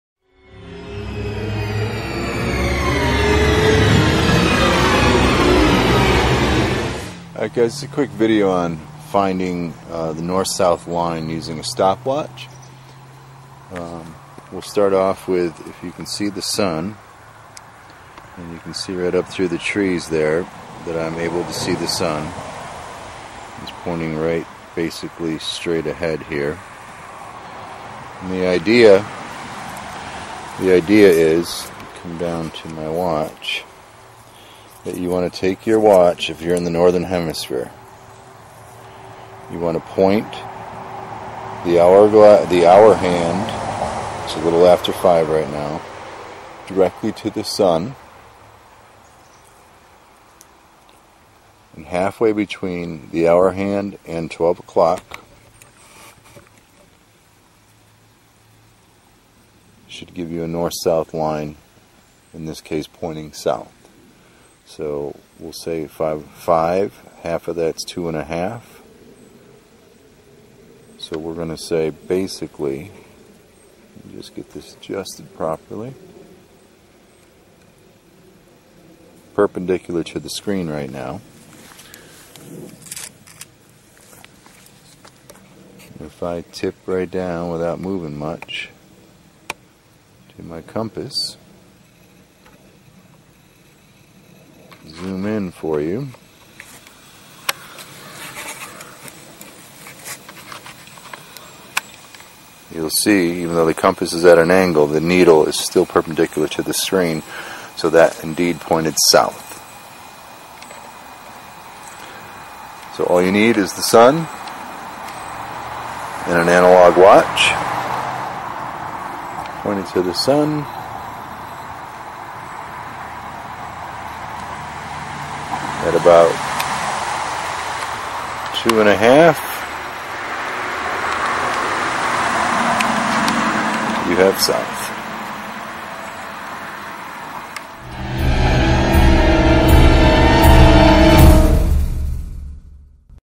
Finding North South Using An Analog Watch On A Sunny Day
This is an example of finding the North South line using an Analog Watch on a sunny day.  You can do this on a cloudy day see my other video